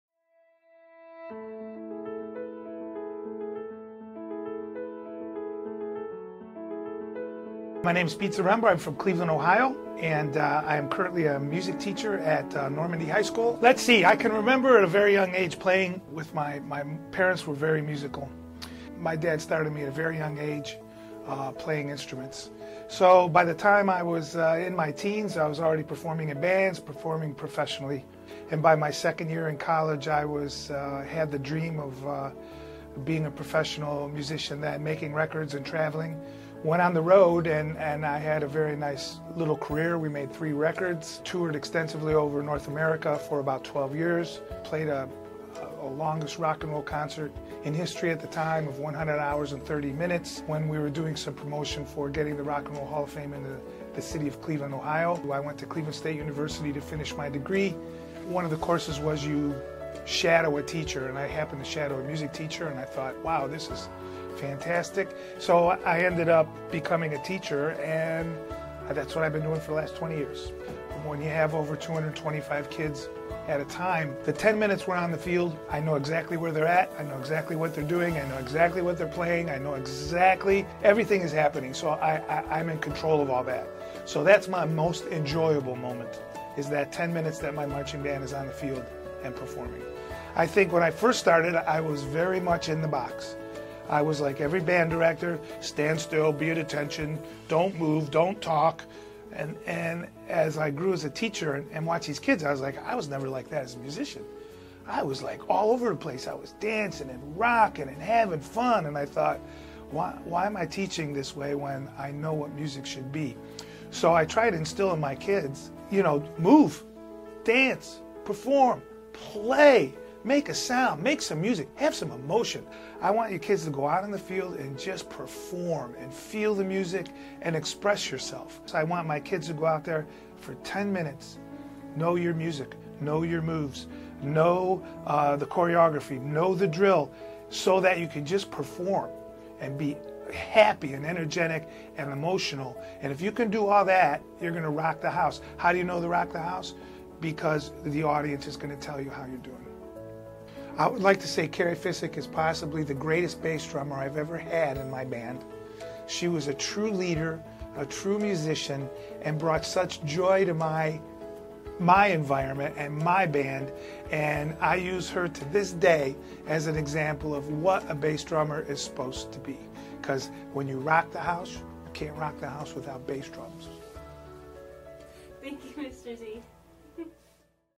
Interview with Mr. Zaremba
Our first project for Production I was to interview someone, so I chose to interview my high school band director! He is a wonderful person with a big heart, and has been such a great mentor in my life.